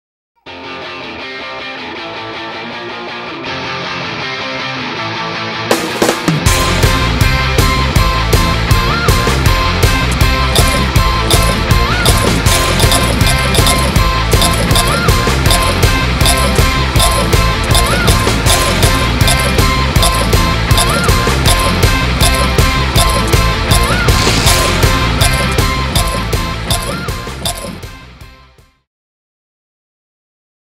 Joues ta Doritos
Doritos procure une énergie autant par son crounch que par son parfum. Une ambiance festive où Doritos pend l\'allure et l\'utilité d\'un instrument, laissant ainsi embarquer tout le groupe dans l\'univers de cette nouvelle saveur!